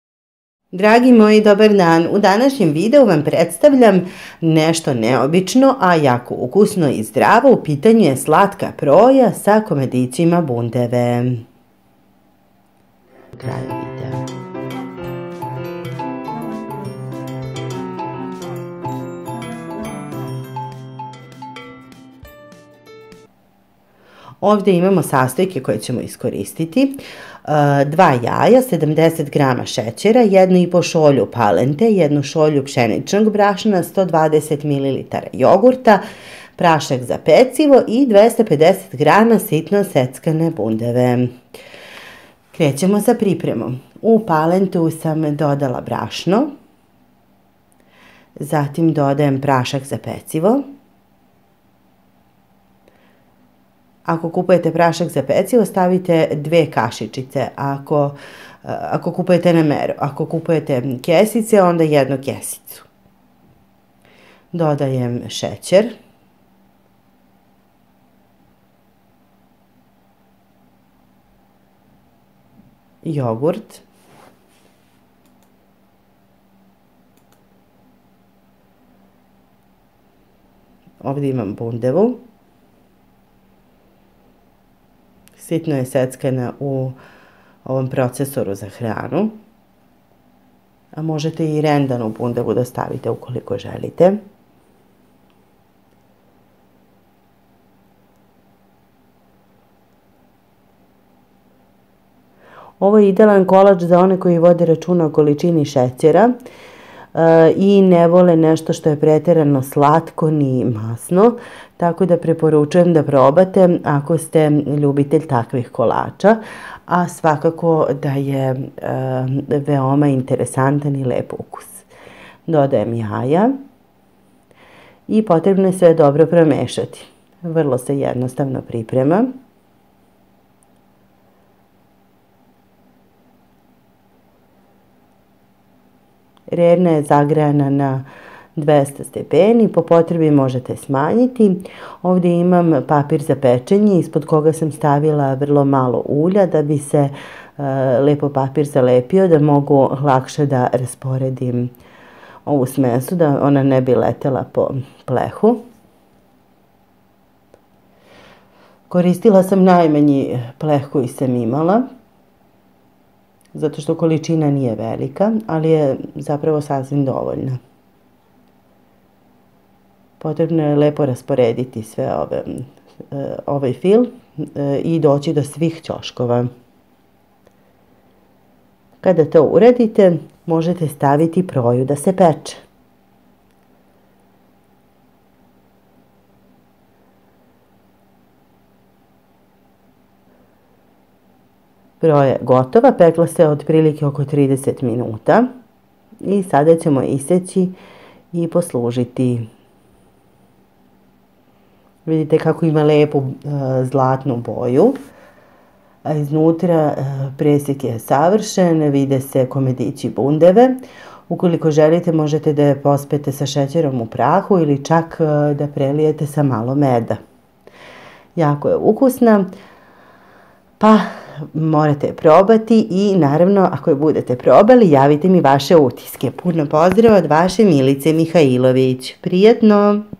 KOLAČ OD PALENTE KOJI OSVAJA | Milica Mihailović
SLATKA PROJA KOJA OSVAJA | Milica Mihailović

Sastojci:
2 jaja
70 g šećera
1.5 šolja palente
1 šolja pšeničnog brašna
120 ml jogurta
2 kasicice praška za pecivo
250 g sitno seckane bundeve